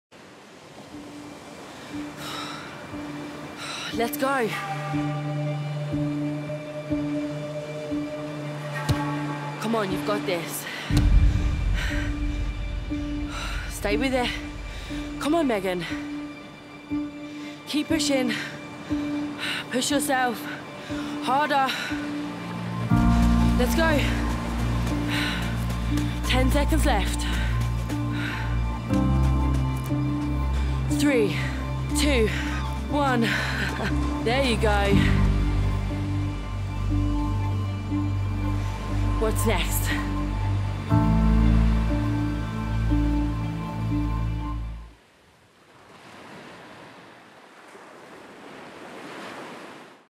Megan Williams
Always

Intersport Elverys have teamed up with Megan Williams Health & Fitness Coach, Irish Women's rugby player and founder of Move Energise Glow to show us how she trains!

Stayed tuned for more videos in this series!